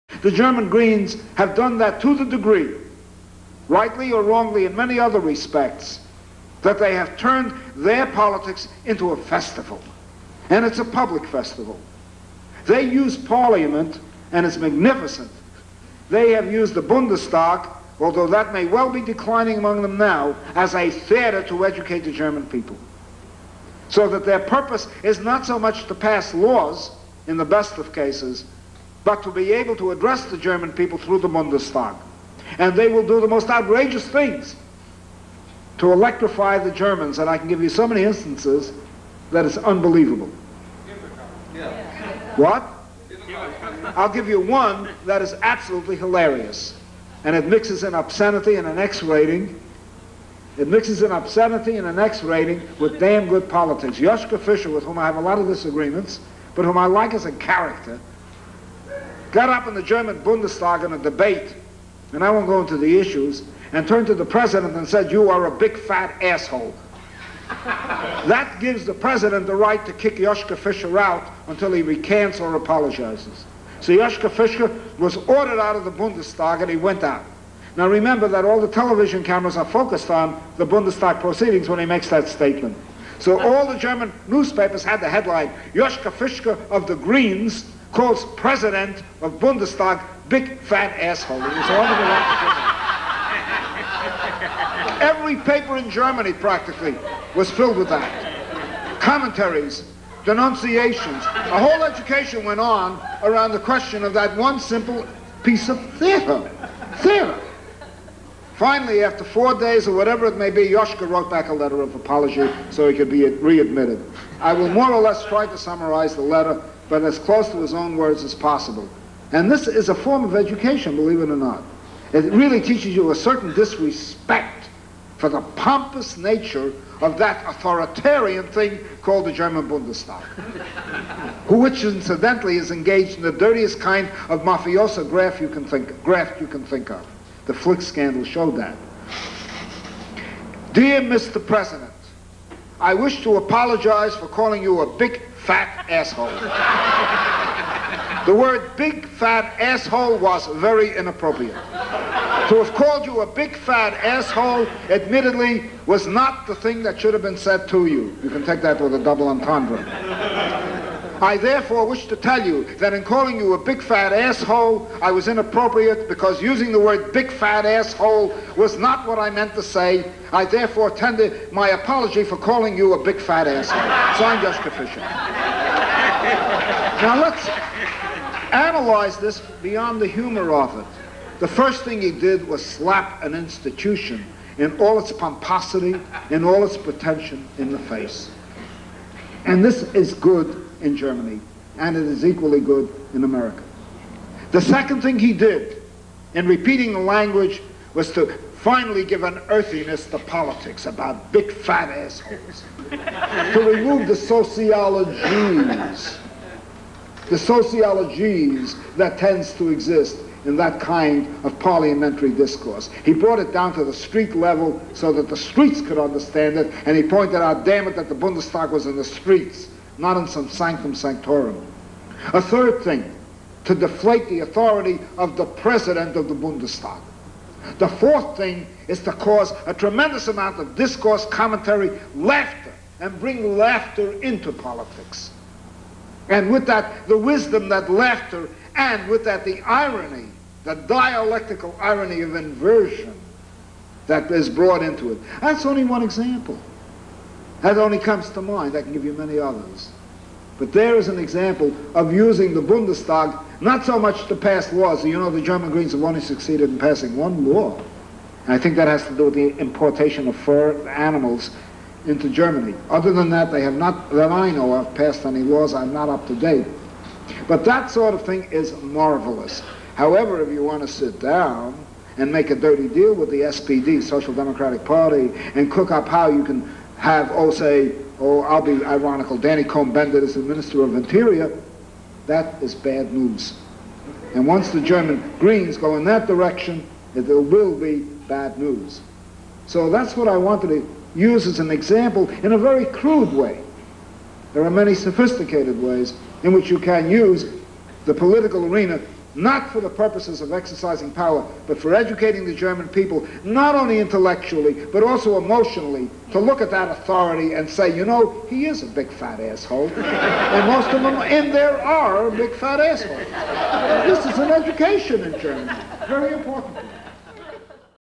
Murray Bookchin on Joschka Fischer calling the Bundestag vice president an asshole
Excerpt from the Q&A portion of Bookchin's "Forms of Freedom" lecture given at The Farm community center in San Francisco, 3/23/85.